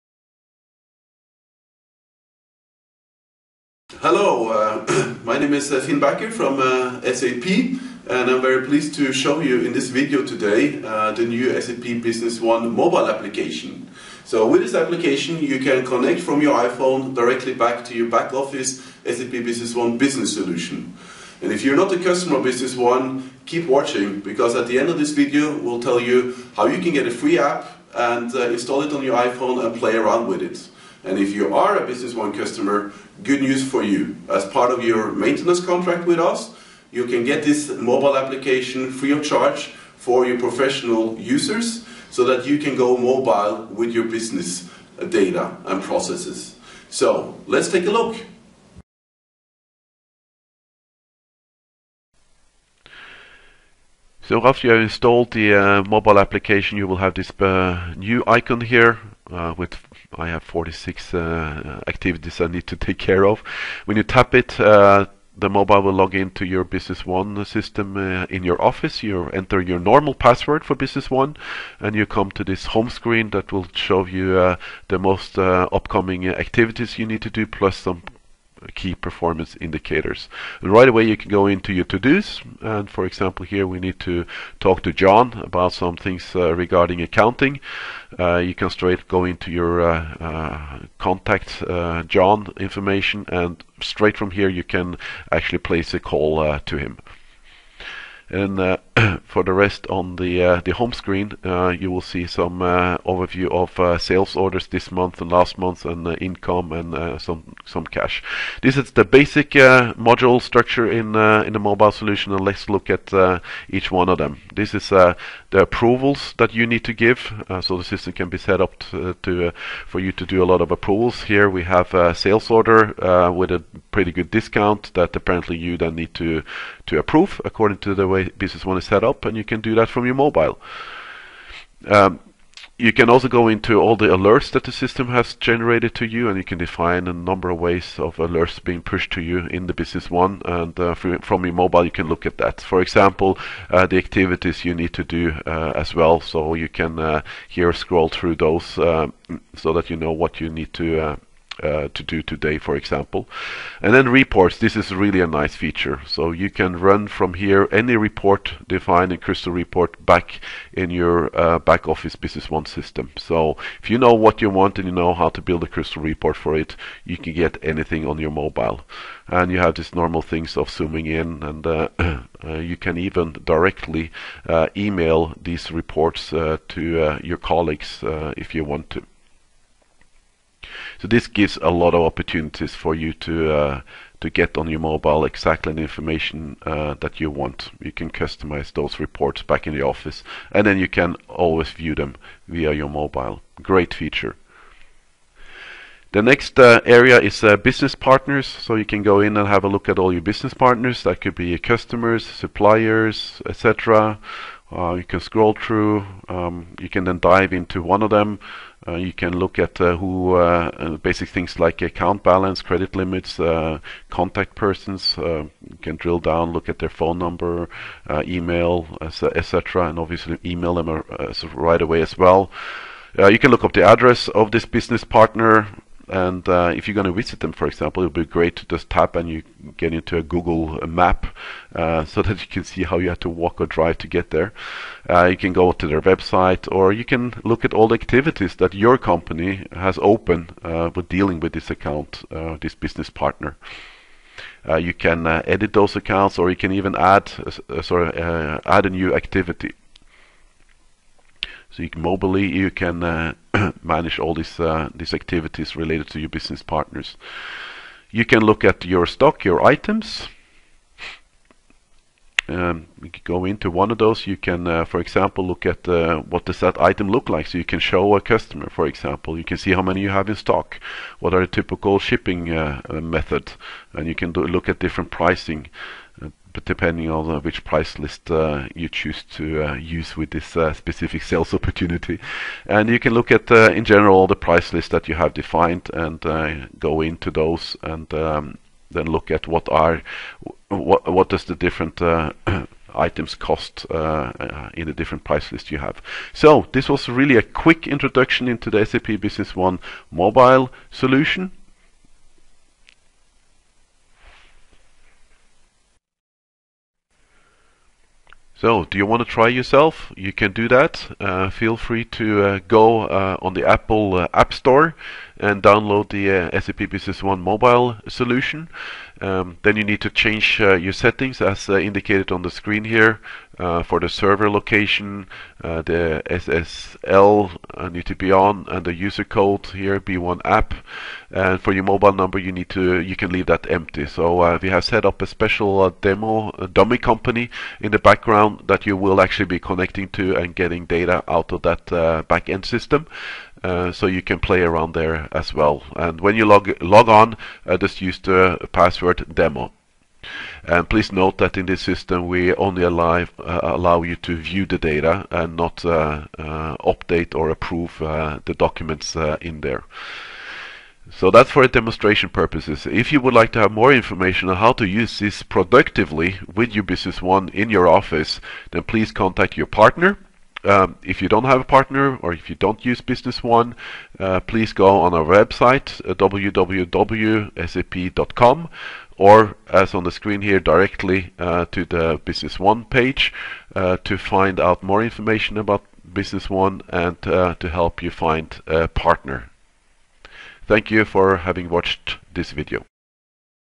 The new SAP Business One iPhone App
In this demonstration, you'll get a quick overview of the new SAP Business One iPhone App which is downloadable now from the App Store